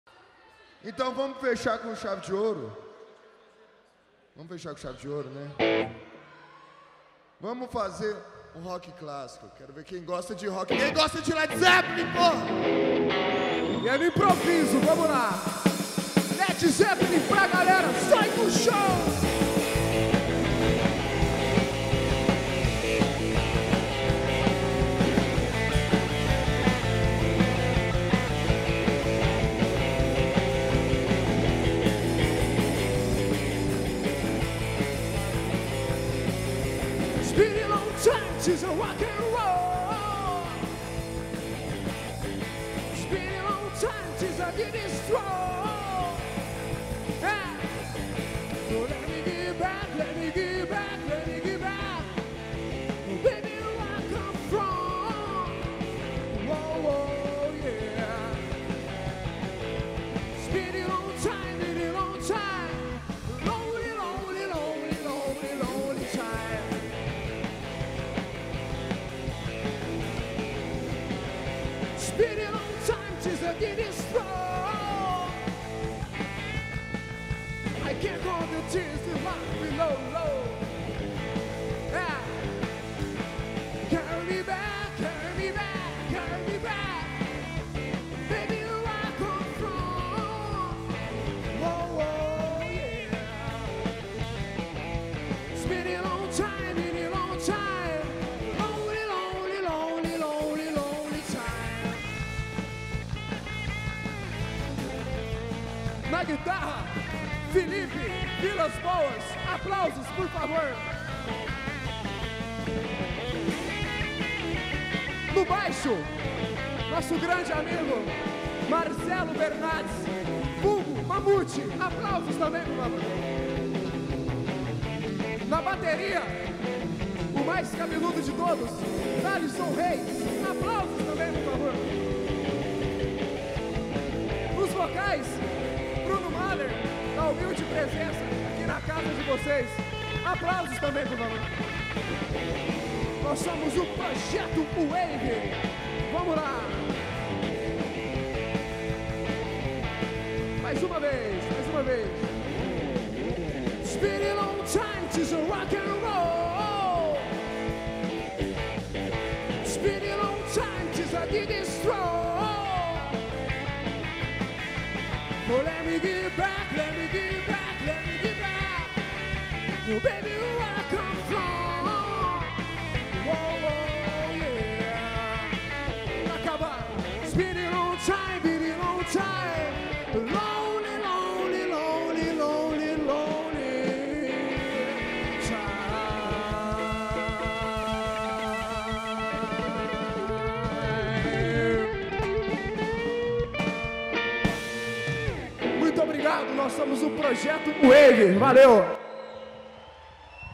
A Banda Projeto Wave na Concha Acústica de Uberaba (Rock And Roll)
Show da Banda Projeto Wave na Concha Acústica, realização Fundação Cultural, no dia 01 de novembro de 2015.

Integrantes:
Bruno Mahler - Vocal e violão
Felipe Vilas boas - Guitarra
Thalison Reis - Bateria
Marcelo Bernardes (Mamute) - Baixo
Edição e imagens: Israel Buzatti
Agradecimentos ao Nilton, por ter cedido o áudio do show.